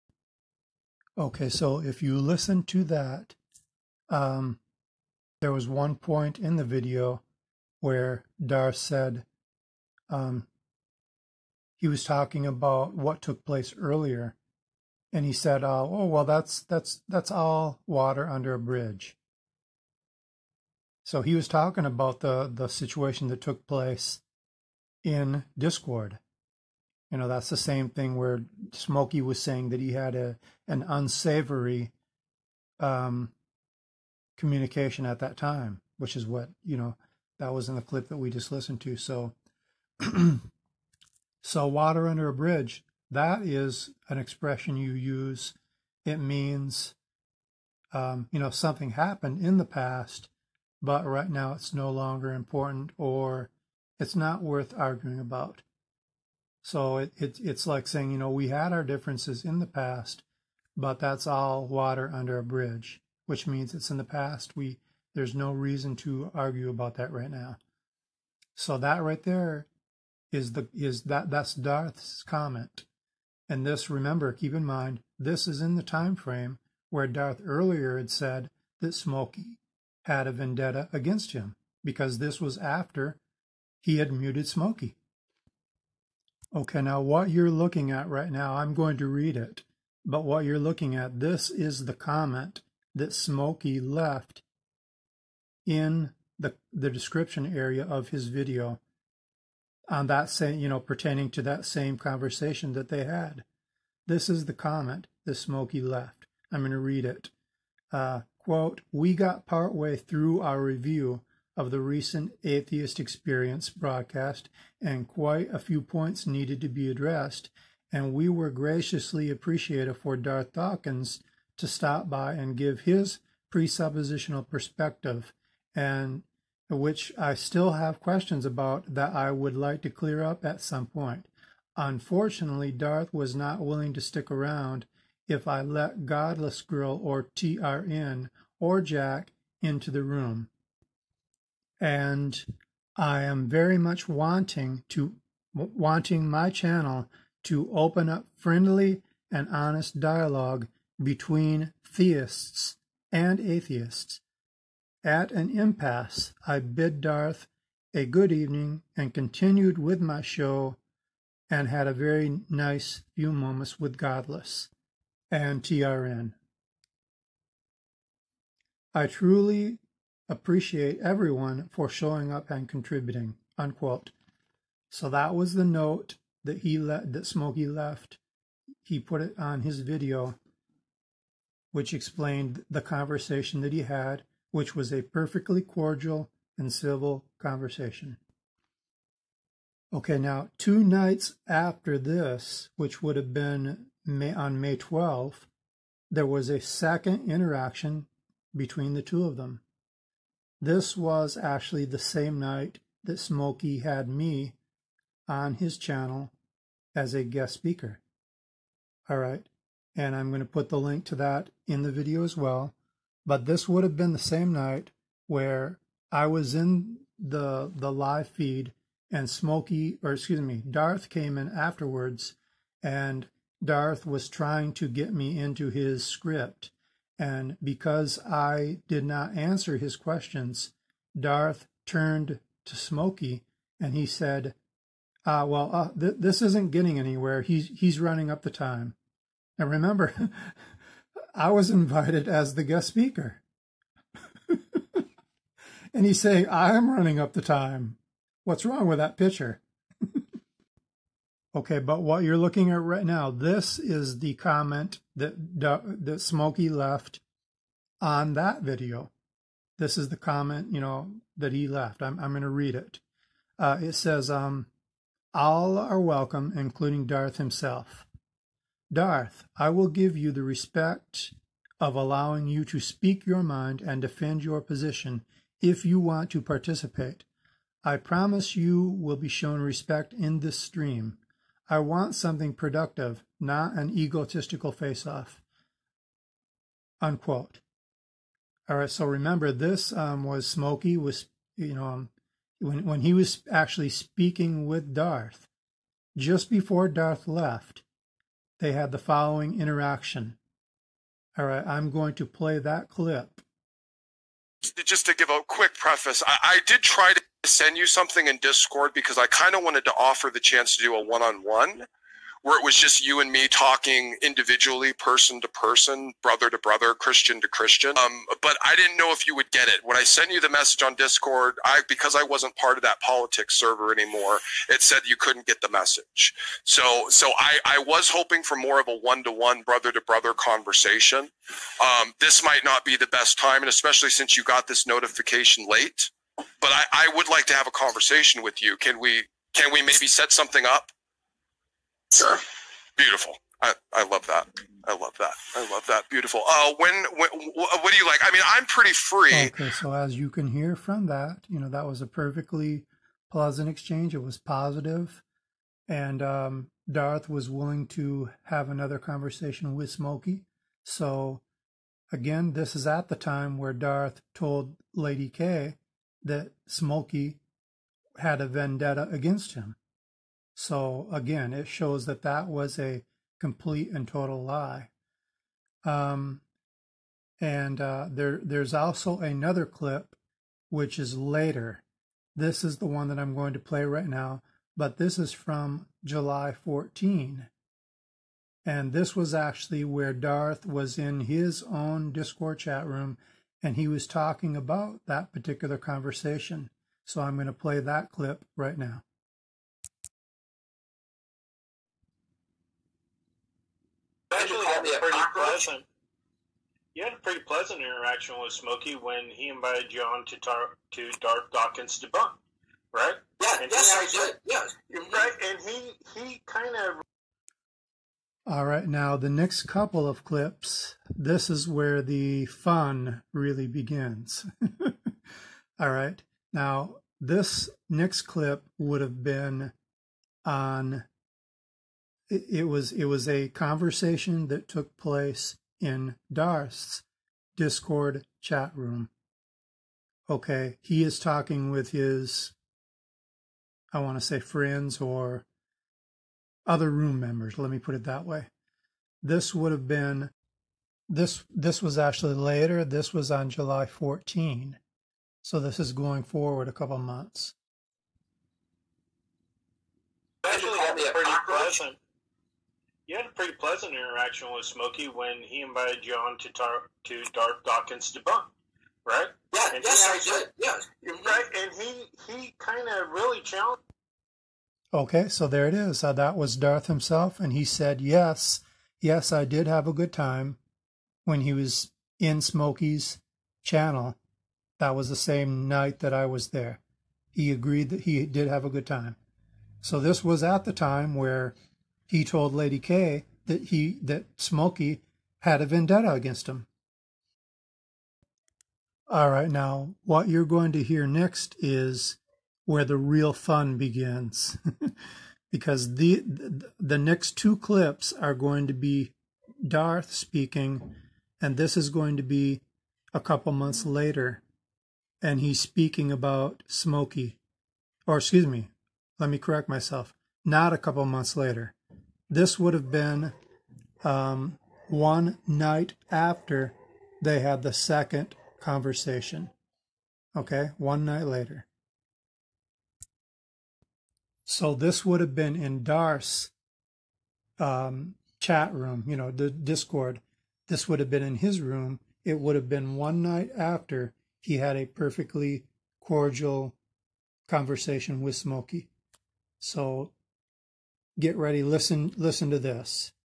Part 2 𝗗𝗮𝗿𝘁𝗵 𝗗𝗮𝘄𝗸𝗶𝗻𝘀: 𝗮 𝗰𝗹𝗼𝘀𝗲𝗿 𝗹𝗼𝗼𝗸 𝗮𝘁 𝘁𝗵𝗲 𝗣𝘀𝘆𝗰𝗵𝗼𝗹𝗼𝗴𝘆 𝗼𝗳 𝗮 𝗰𝗼𝗺𝗽𝘂𝗹𝘀𝗶𝘃𝗲 𝗟𝗜𝗔𝗥. 𝗦𝗲𝗲 𝘃𝗶𝗱𝗲𝗼 𝗱𝗲𝘀𝗰𝗿𝗶𝗽𝘁𝗶𝗼𝗻
This is Part 2 of 4. The video Clip where Smokey says he wants to do a one on one chat with Darth and Darth agrees to it

Link to 29 minute conversation between Darth and Smokey

Link where Darth agreed with a member of his own Discord chatroom that him and Smokey had a pleasant conversation with each other, which is in the same time frame where Darth told Lady K that Smokey was on a "vendetta" against him.
Darth says (according to his own standard) that "...𝐥𝐢𝐚𝐫𝐬 𝐚𝐫𝐞 𝐰𝐨𝐫𝐬𝐞 𝐭𝐡𝐚𝐧 𝐩𝐞𝐝𝐨𝐩𝐡𝐢𝐥𝐞𝐬/𝐜𝐡𝐢𝐥𝐝 𝐦𝐨𝐥𝐞𝐬𝐭𝐞𝐫𝐬...", but can you think of a bigger BOLD FACED LIAR  than Darth Dawkins?